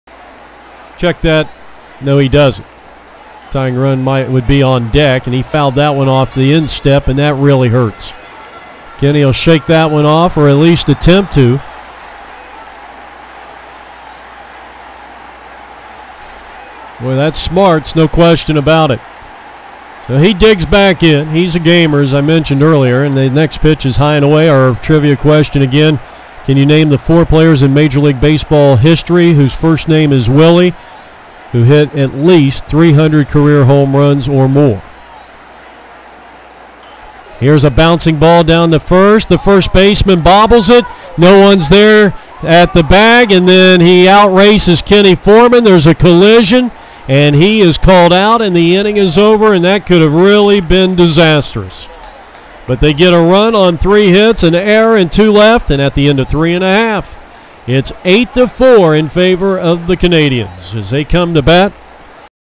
2007 MSBL World Series - "That could have really been disastrous ..."
Randy Enns of the Komox Marine Harvest Canadians gets some fifth inning help from Fabian McCarty against Michigan RIT Music at the 2007 MSBL World Series in Phoenix, AZ.